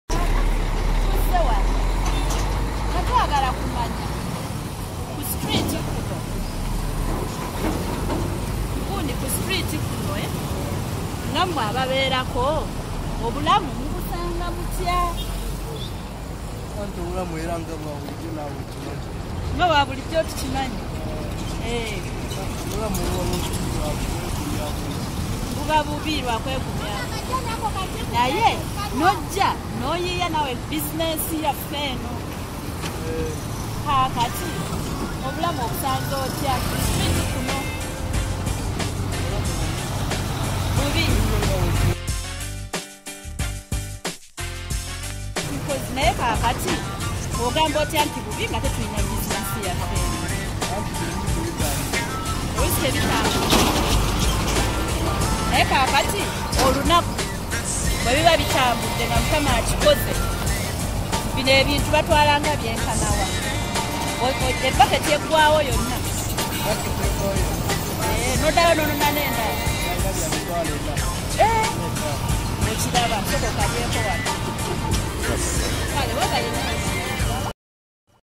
Mukono Street Vendor - Street Life
Video identifying the issues that confront street vendors in the town of Mukono, Uganda. He identifies the viability of his informal bagiya business.